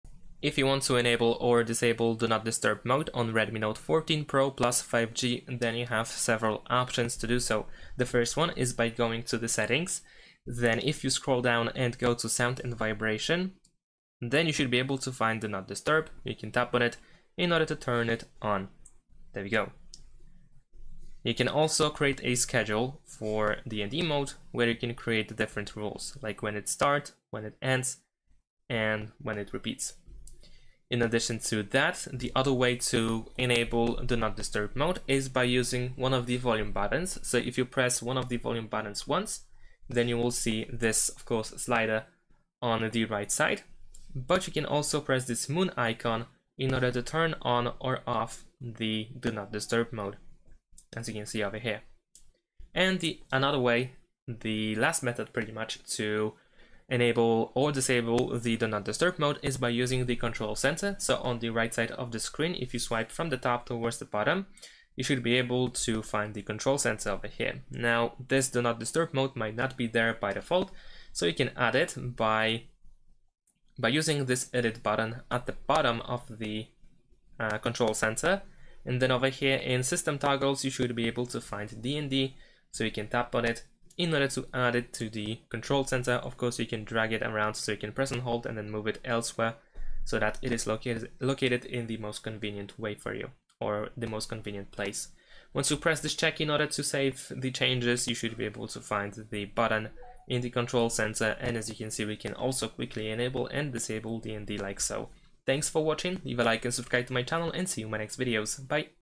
Redmi Note 14 Pro+ 5G: How to Turn ON/OFF Do Not Disturb Mode (DND Mode)
Learn how to enable or disable the Do Not Disturb (DND) mode on your Redmi Note 14 Pro+ 5G in this easy-to-follow tutorial. Whether you want to silence notifications for a focused experience or turn them back on, this guide covers all the steps. Perfect for managing interruptions during meetings, sleep, or gaming sessions. Watch now to master the DND settings on your Redmi smartphone. Simplify your smartphone usage with these quick tips!